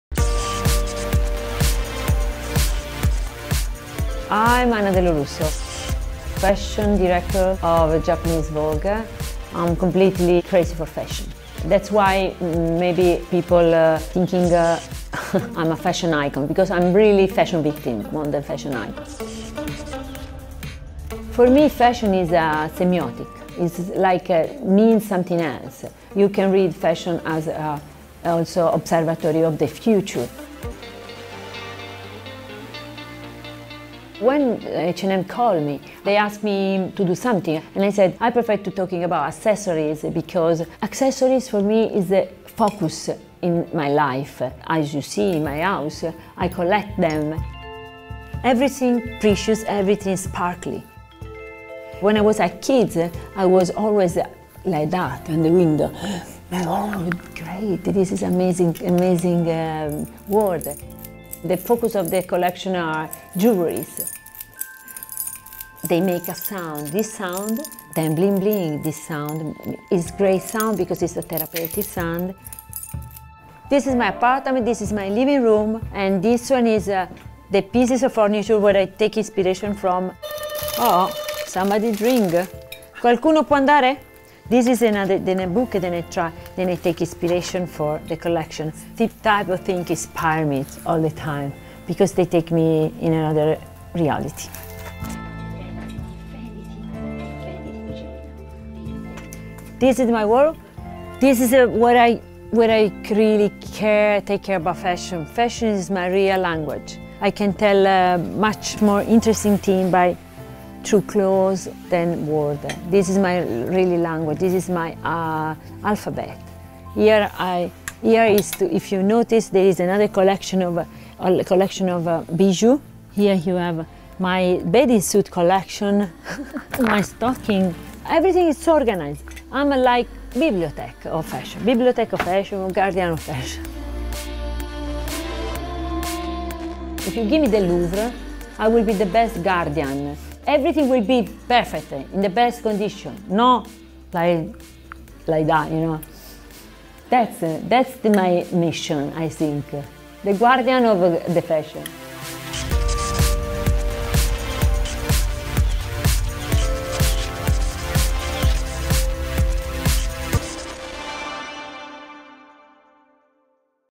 Anna Dello Russo accessories collection for H&M
Anna Dello Russo is set to design an over-the-top accessories collection for H&M. The line will be available from October 4, 2012, at the peak of the global fashion weeks tour, in around 140 stores worldwide and online. The collection features jewellery, sunglasses, shoes, bags and a trolley allowing H&M customers uniqueness at an affordable price.